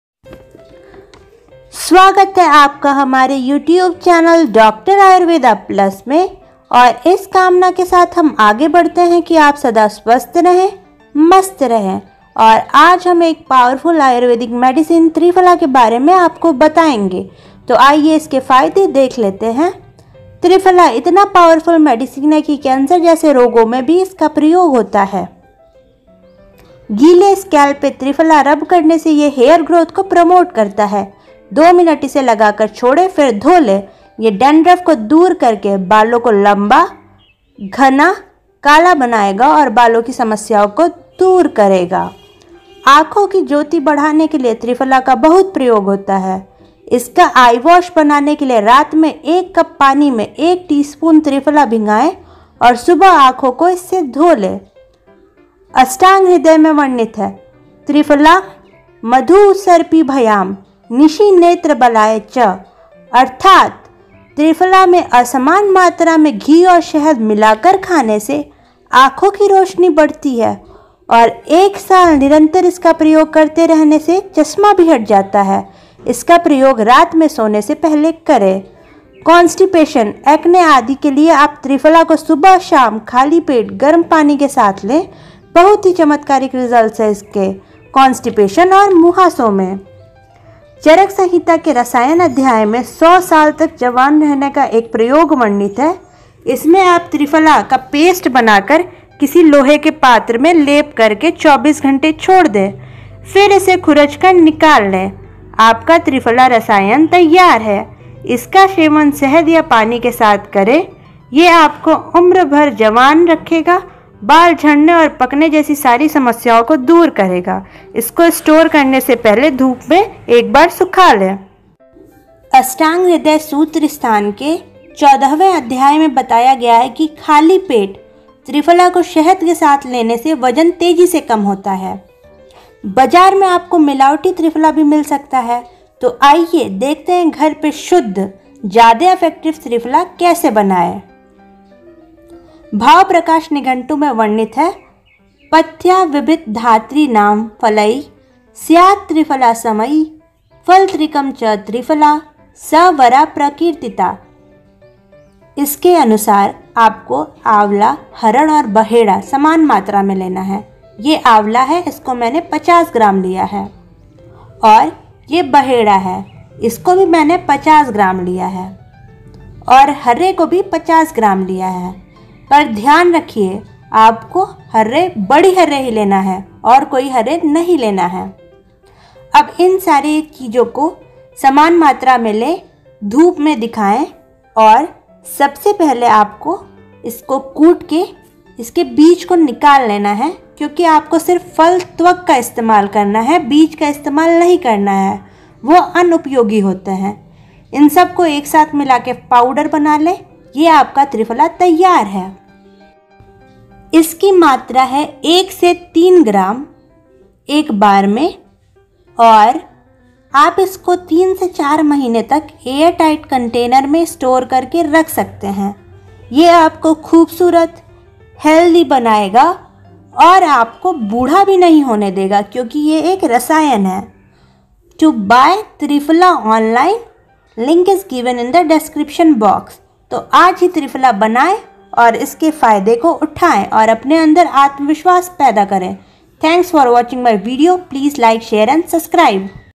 Powerful ayurvedic medicine to become beautiful and healthy forever||Triphala Rasayan
MostPowerfulMedicineOfAyurveda
Cancer
Hairgrowth
ImprovesEyesight
RemovesSpectacles
VitaminC
Acne
Constipation
Obesity

Recipe of triphala according to Bhavprakash Nighantu Haritakyadi Varga 42
Indian gooseberry (Amla), Terminalia chebula(Harre),Terminalia bellirica(Baheda)

For method of preparation  see video

Dosage of triphala churna:adult 1 to 3gm

To buy triphala  powder online